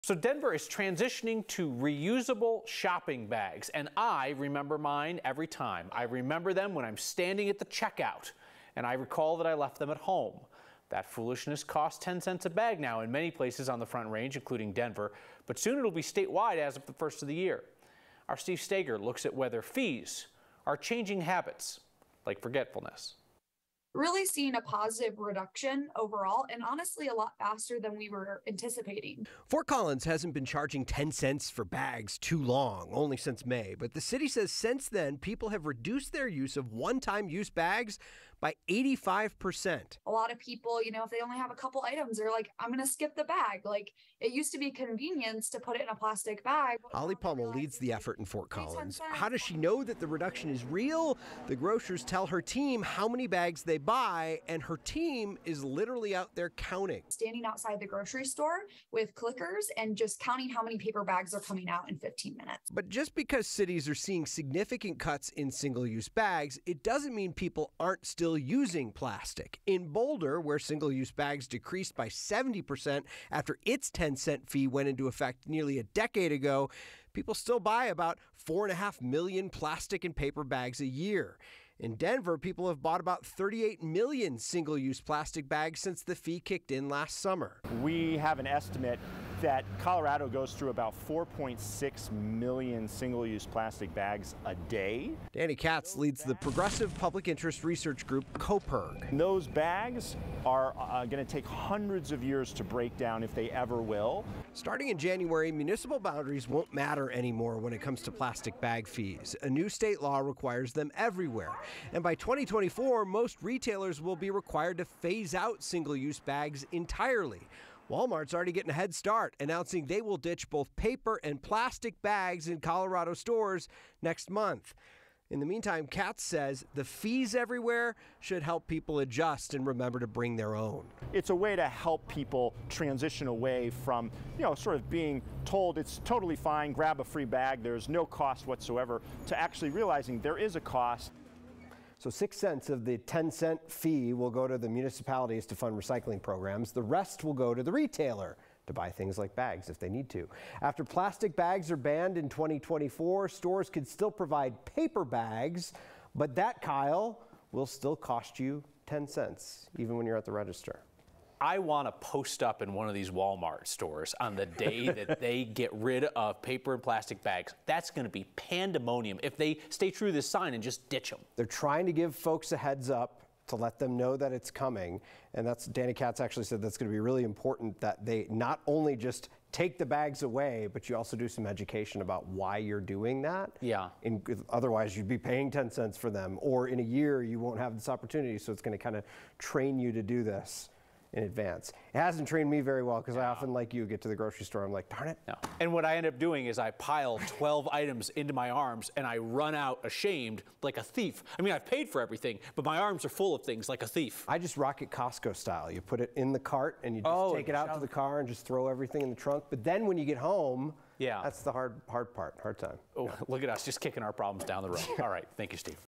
Plastic bag fees coming to Colorado statewide Jan. 1
With each bag costing you an extra 10 cents at checkout, are bag fees changing habits?